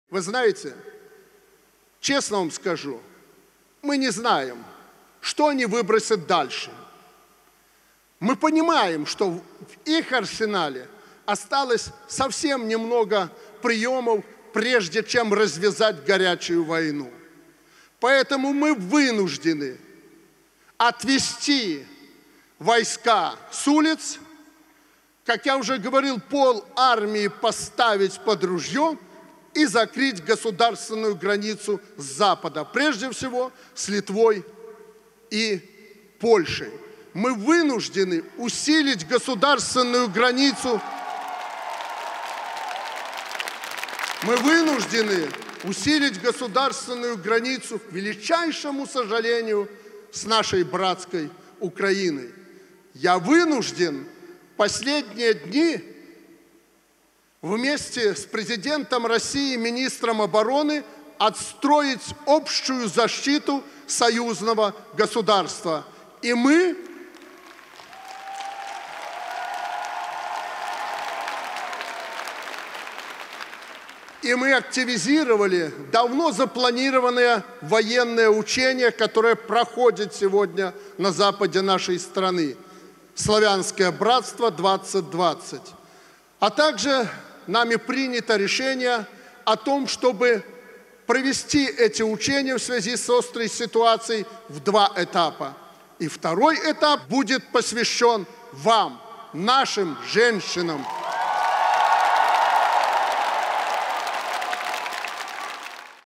Лукашенко рассказал, что ситуация вынудила вместе с Россией отстроить защиту Союзного государства
Президент Беларуси Александр Лукашенко рассказал, что сложившаяся ситуация вынудила вместе с Россией отстроить общую защиту Союзного государства. Об этом он заявил, выступая в Минске на женском форуме "За Беларусь".